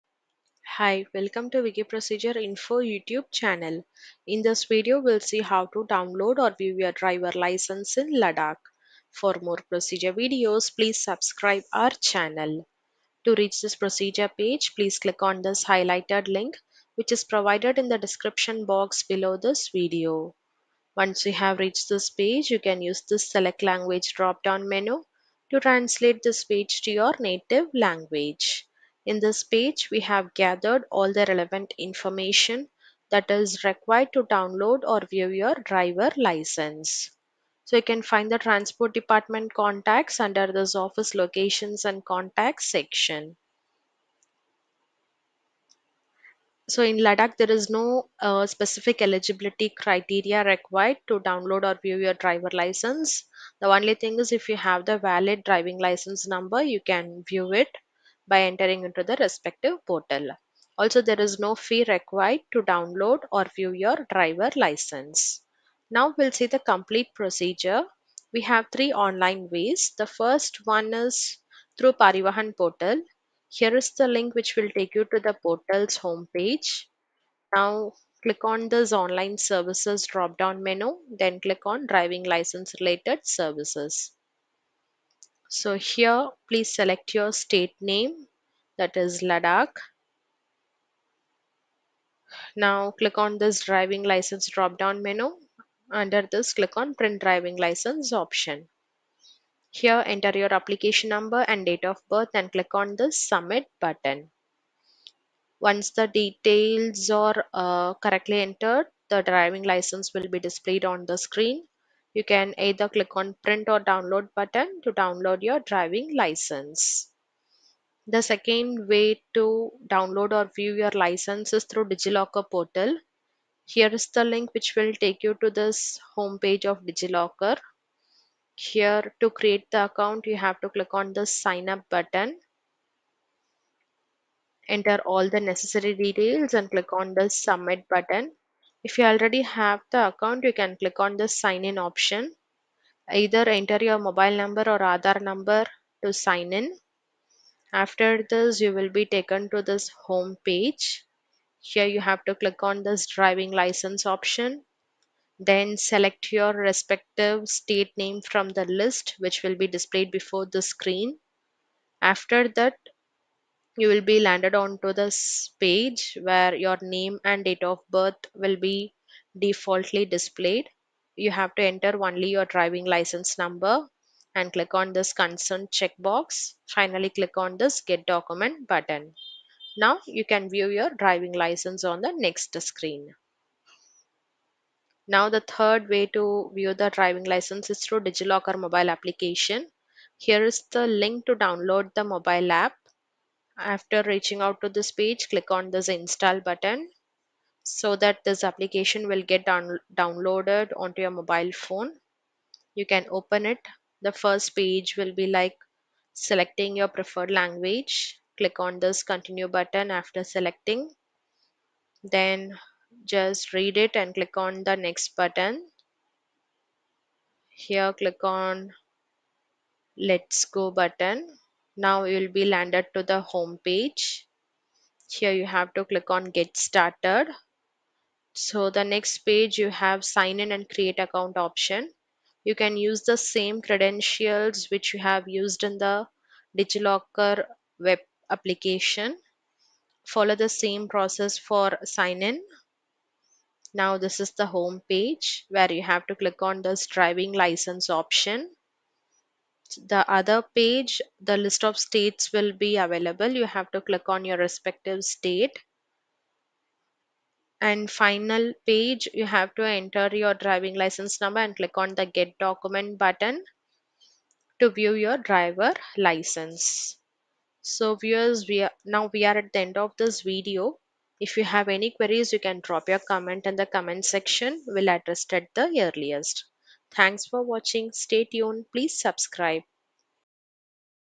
Ladakh - Download or View Driver License (Online)
This video provides you information related to Download or View Driver License in Ladakh. Following link can take you to the page with more information - Download or View Driver License#_